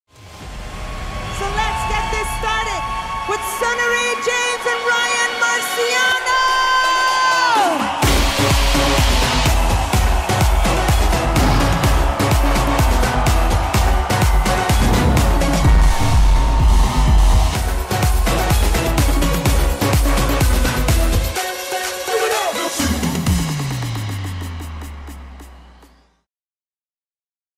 Sunnery James & Ryan Marciano | Mysteryland 2017
Relive Sunnery James & Ryan Marciano playing at our magnificent Main Stage at Mysteryland 2017. Get a taste of how this energetic duo seamlessly blended House, Tribal and Techno into their unforgettable set!

Early Bird tickets for Mysteryland 2018 will go on sale on February 27th at 12h (CET). Mark your calendars!